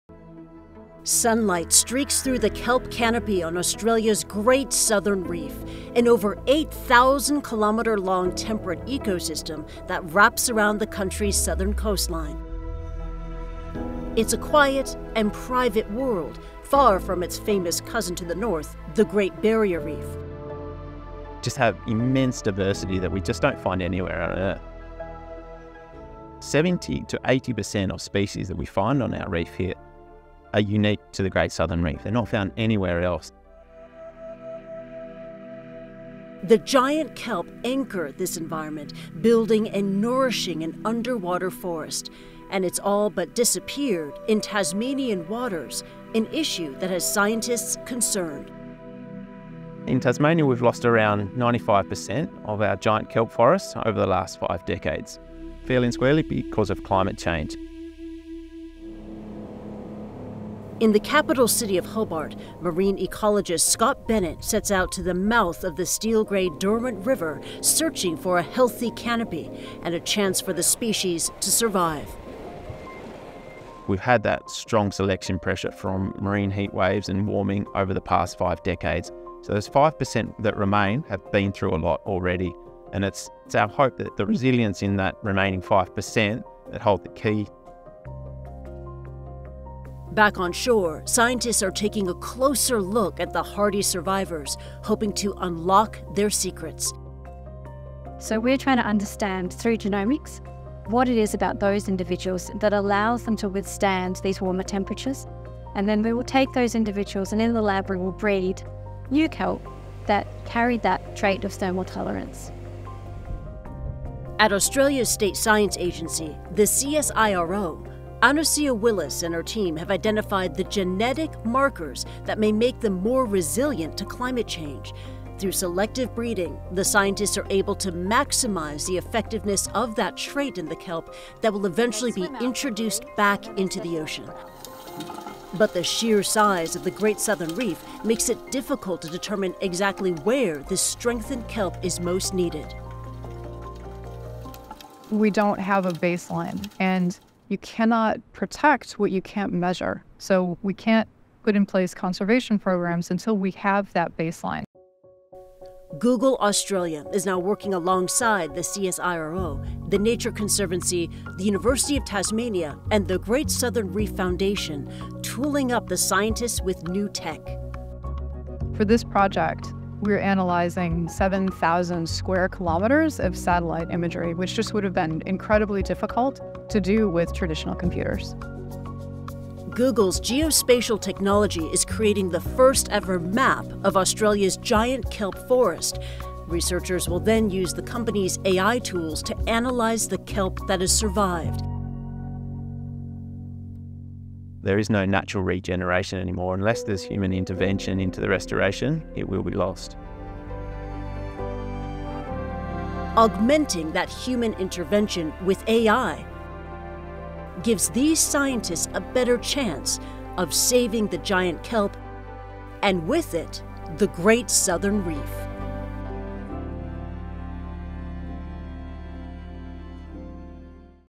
علماء يستخدمون الذكاء الاصطناعي لحل مشكلة اختفاء "غابات بحرية" في أستراليا
كانت منطقة الحاجز المرجاني الجنوبي العظيم في أستراليا، وهي عبارة عن نظام بيئي معتدل يمتد إلى أكثر من 8000 كيلومتر، تعجّ بالأعشاب البحرية العملاقة، التي تتمتّع بتنوّع هائل لا يوجد في أي مكان آخر على وجه الأرض، لكنها اختفت تقريبًا. وفي الآونة الأخيرة، بدأ باحثون في استخدام تكنولوجيا متقدّمة لإعادة زراعة هذه الغابات تحت الماء وترميمها للمستقبل.